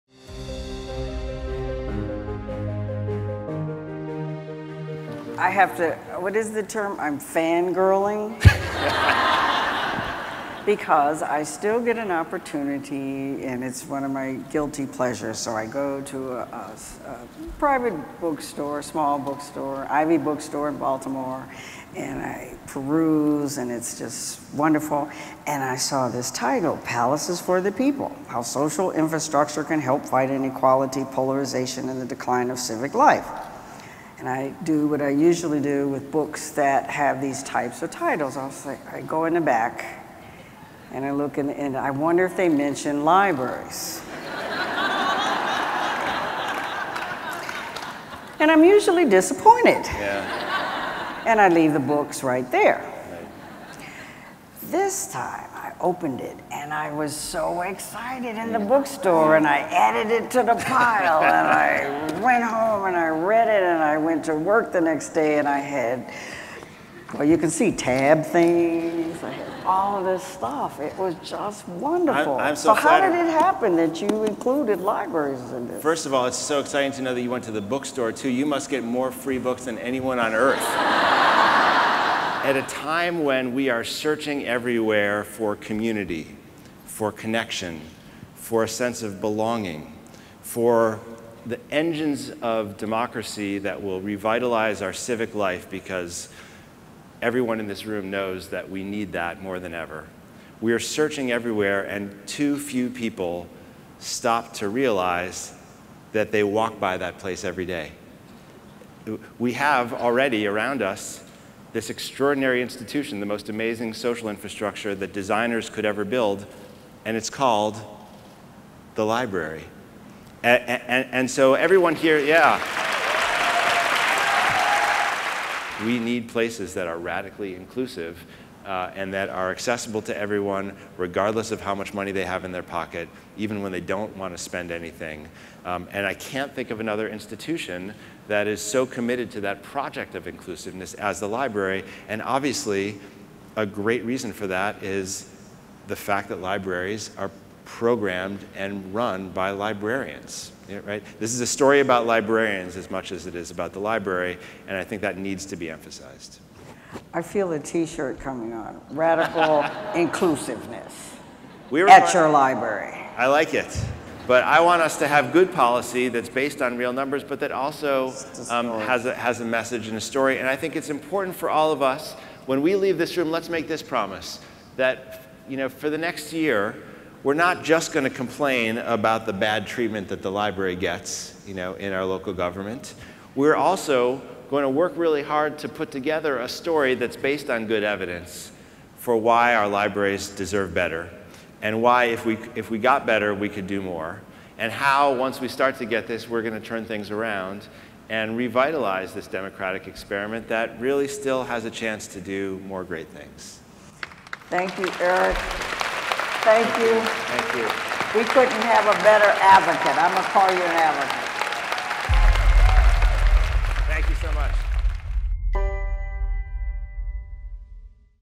ALA Annual 2019 - Hayden, Klinenberg on Libraries, Radical Inclusiveness
Explore the Adventures of ALA Annual in Washington DC!